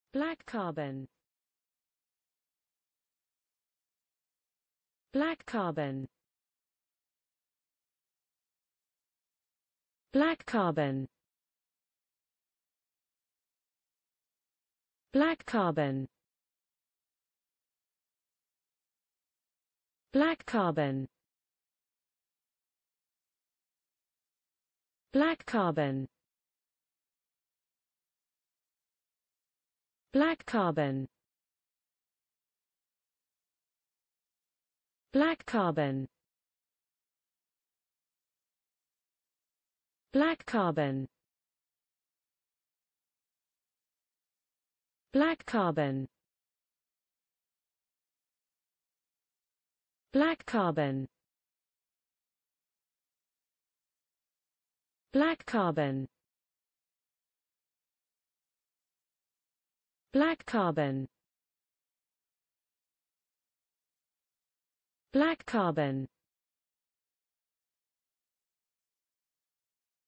Black Carbon: How to pronounce Black Carbon in english (correct!).Start with B. Learn from me.
Follow me!Learn from me how to pronounce the word Black Carbon in english.This video shows you how to pronounce the word Black Carbon in english.

Black Carbon

化工 炭黑